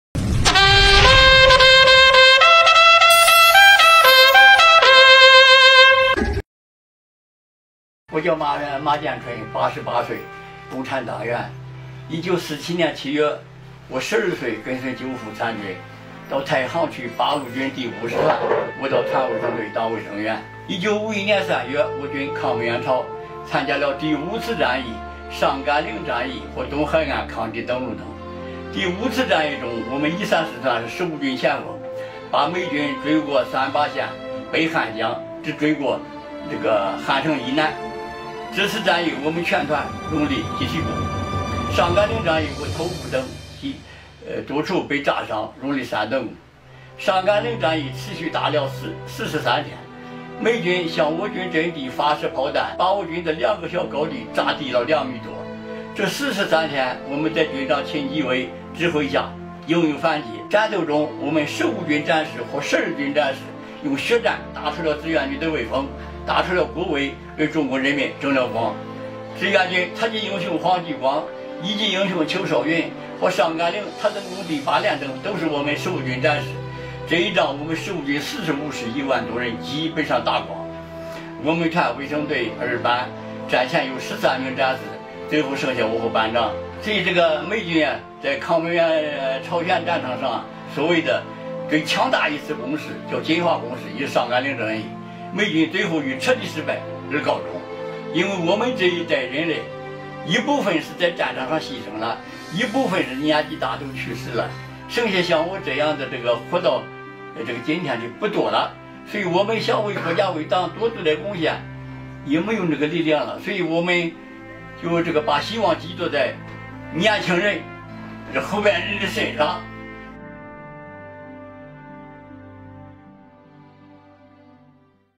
纪念抗美援朝战争胜利70周年只剩3人仍坚持战斗，他们用血战打出了志愿军的威风
马建春12岁入伍，1951年3月赴朝成为卫生队一员。时过境迁，留在他记忆中的仍是当年战斗的惨烈，是没能一起回来的战友。戳↓↓听抗美援朝老兵的战斗青春！